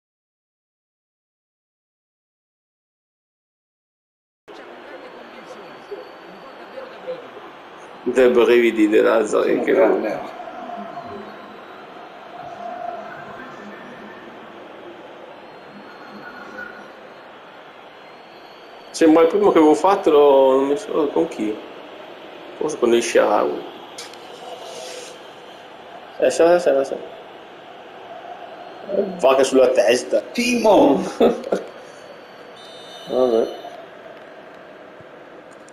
Timo Werner rovescia sulla testa di De Vrij
Timo segna un gran gol in rovesciata...ma la testa di De Vrij e' salva.. :))